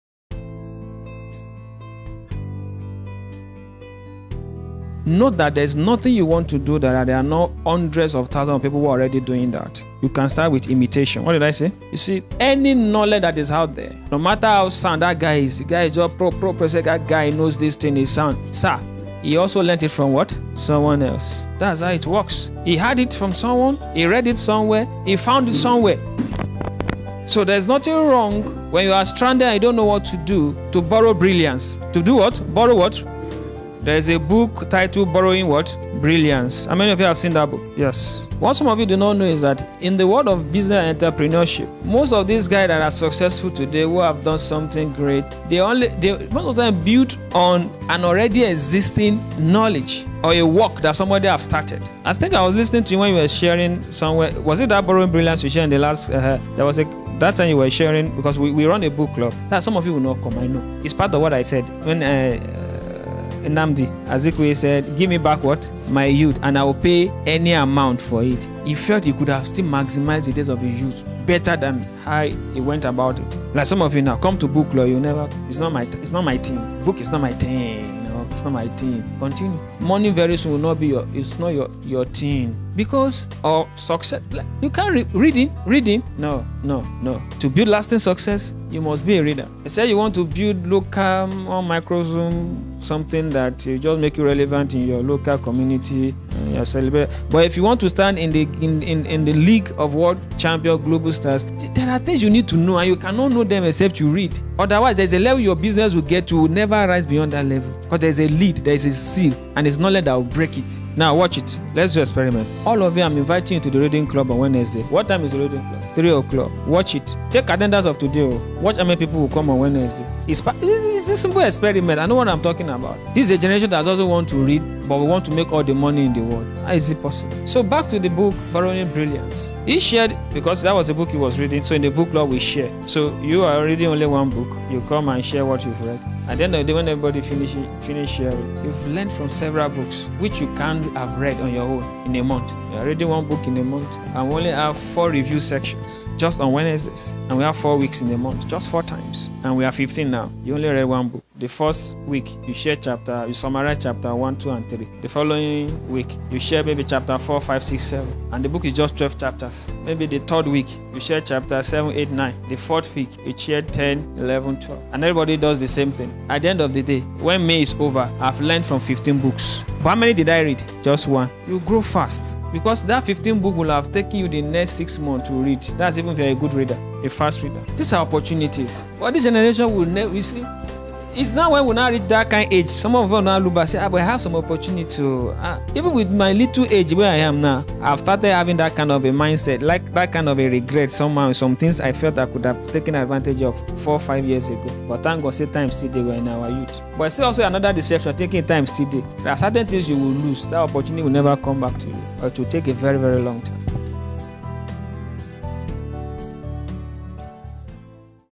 Spark. Innovate. Thrive: How Knowledge Ignites Breakthroughs
In this video, we dive deep into the fascinating dance between innovation, imitation, and knowledge. We'll explore how imitation is often the stepping stone to groundbreaking ideas, and how knowledge is the fuel that propels us forward.

But here's the secret weapon: books! That's right, reading unlocks a treasure trove of knowledge from the world's greatest minds. By immersing yourself in the works of others, you gain the foundation to build upon, the inspiration to ignite your creativity, and the tools to transform ideas into reality.

So, buckle up and get ready to unlock your innovative potential! Let's crack open some knowledge and watch your ideas take flight!